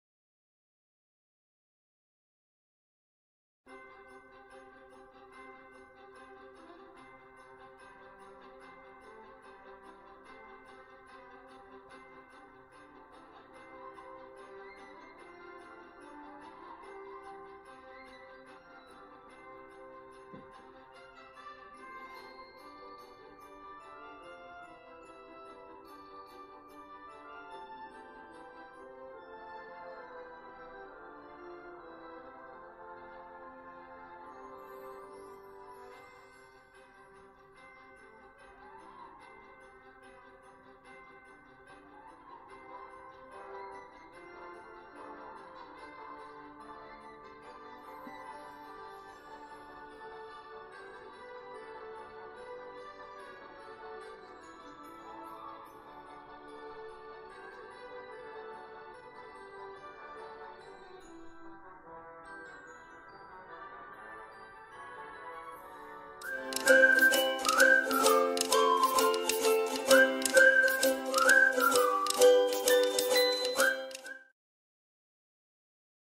How to make an easy paper Christmas tree 🎄 | Christmas craft | easy paper craft for kids
In this video I will show you how to make an easy paper Christmas tree. This little craft was so easy to set up.
You will only need green paper and a paper straw. (I used playdoh for the Christmas tree stand)